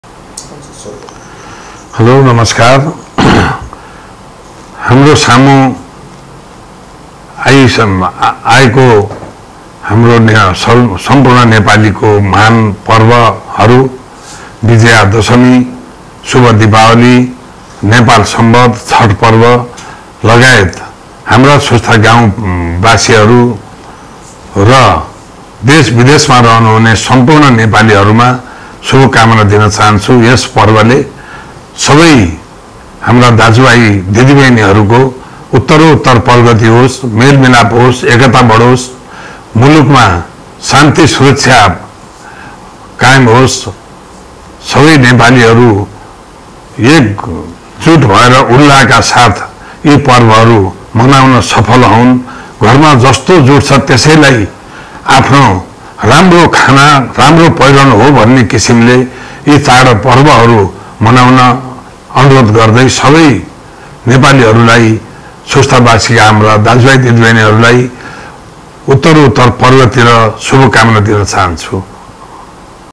शुभ बिजया दशमी , दिपावली एवं छठ पर्वकाे हार्दिक शुभकामना : टेक नारायण उपाध्याय
शुभ बिजया दशमी , दिपावली एवं छठ पर्वकाे हार्दिक शुभकामना :  टेक नारायण उपाध्याय    ( अध्यक्ष   , सुस्ता गाउँपालिका    )